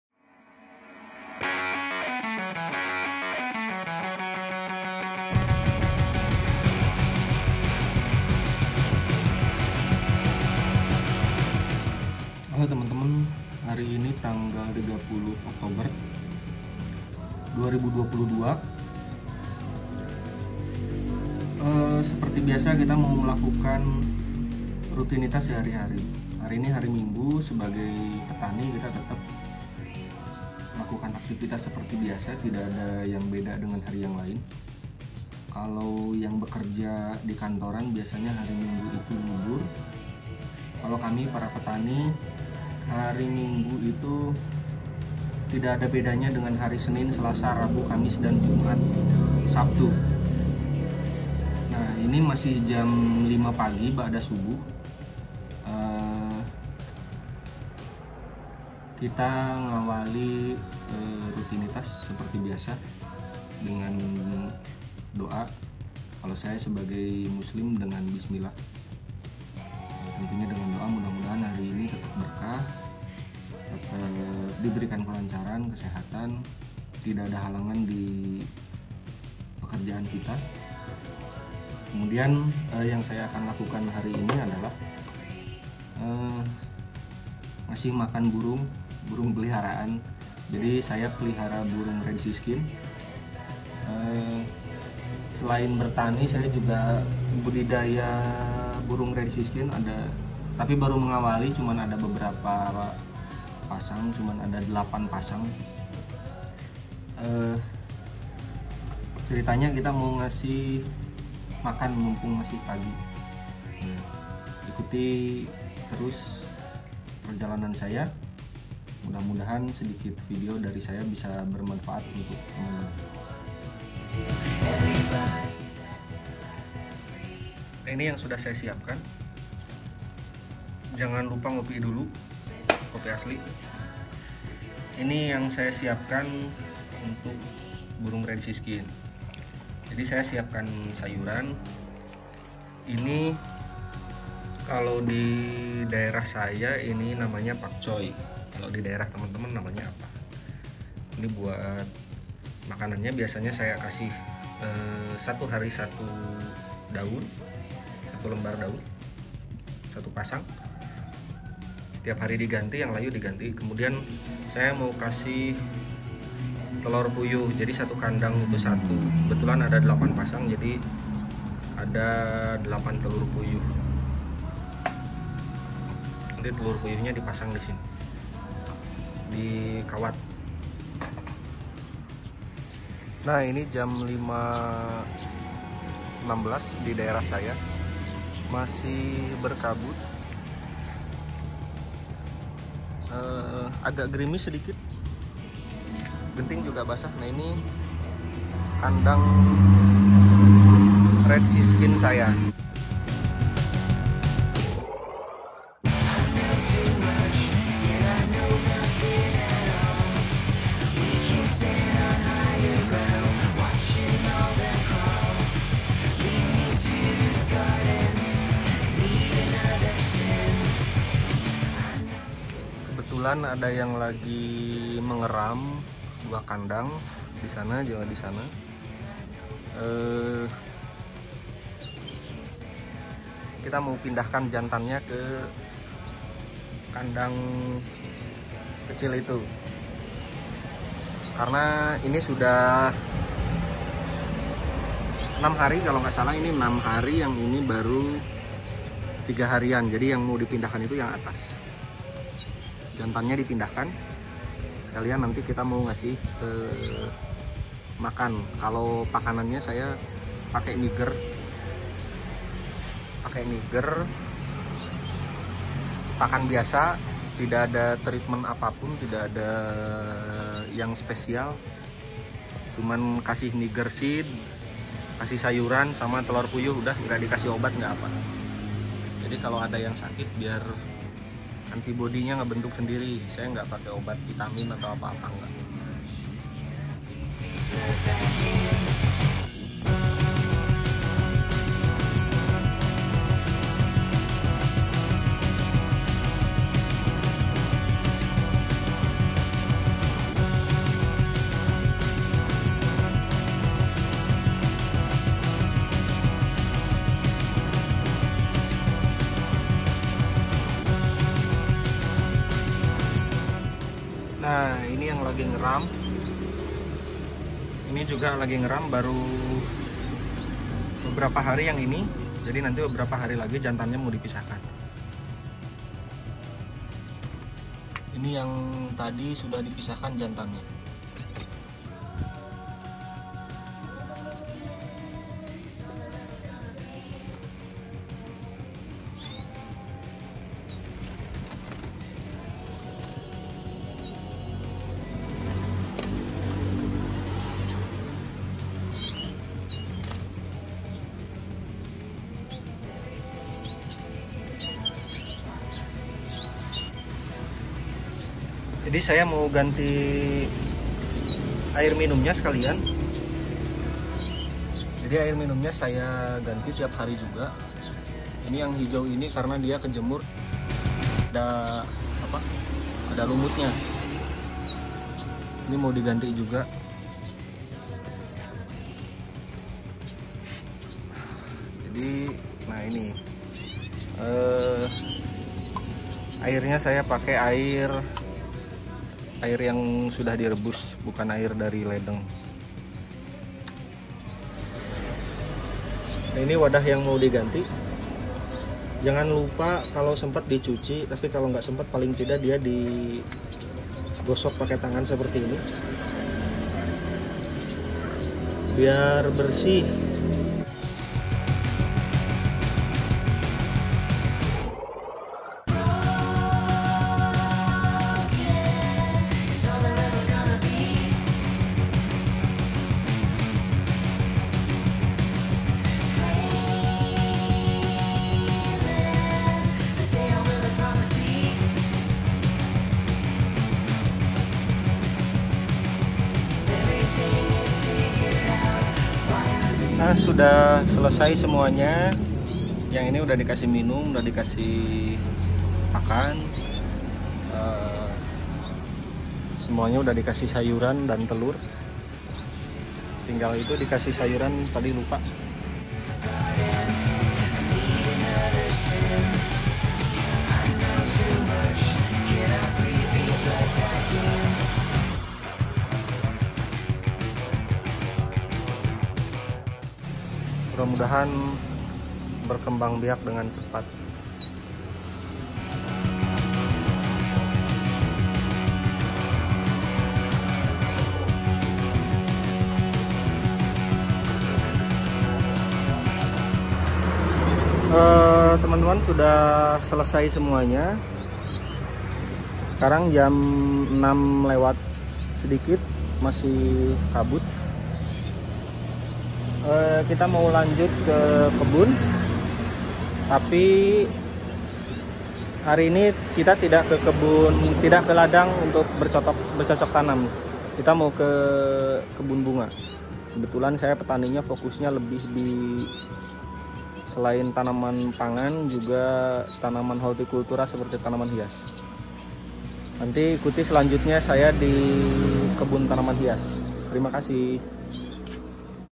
A day in my life. kegiatan kami sebagai petani milenial, jadwal ngasih makan burung red siskin
Sekain bertani ke kebun setiap hari ada kegiatan lain sebagai hiburan yang mungkin nanti akan jadi cuan juga hehe ngasih makan burung red siskin, perawatan burung res siskin, tips pakan burung red siskin.
Jangan malu jadi petani